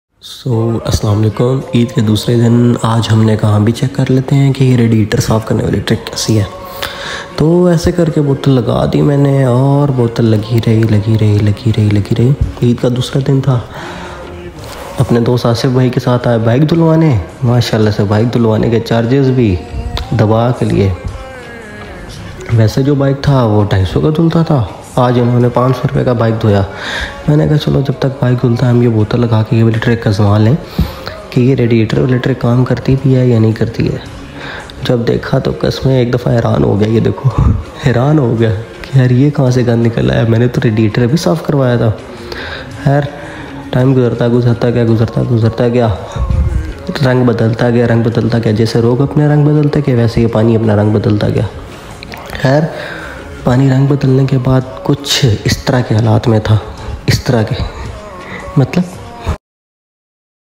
Tested Free Radiator Cleaning Trick, It Works😳😳
Just tried to check if that trick works or not and I’m shocked to see the results